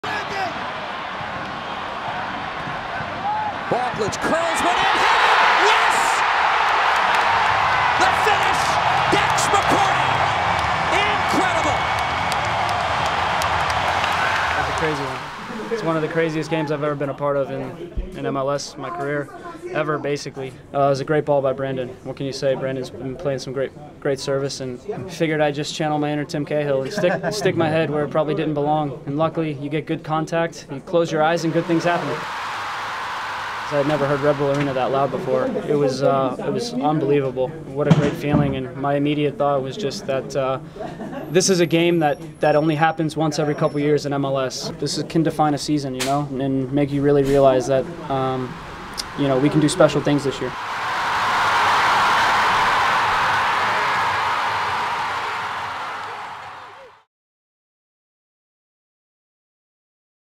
Dax's Magic
Dax McCarty powered the Red Bulls to three points over RSL thanks to a little last-minute magic